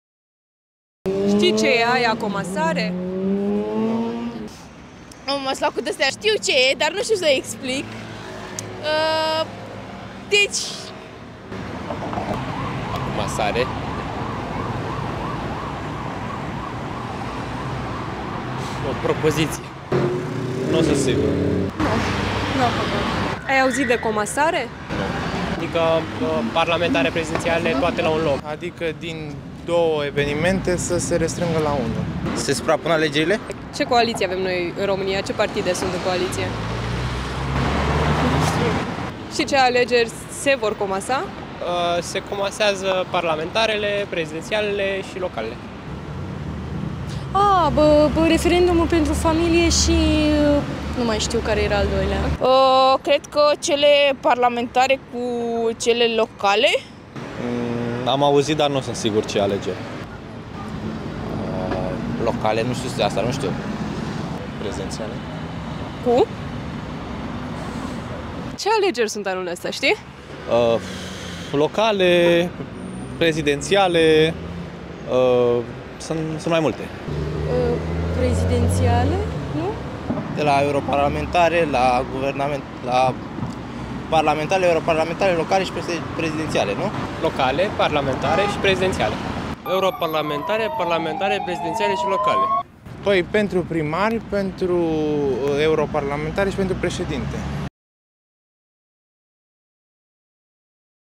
Ce știu tinerii români despre comasarea alegerilor?
În anul super-electoral 2024, mulți tineri nu sunt siguri când și pentru ce vor vota. Întrebările „ce este comasarea?” și „ce alegeri vor fi organizate anul acesta” îi pun în încurcătură.

În ciuda acestui fapt, câțiva tineri din București sunt hotărâți să meargă la alegerile de anul acesta, chiar dacă nu știu care sunt acelea.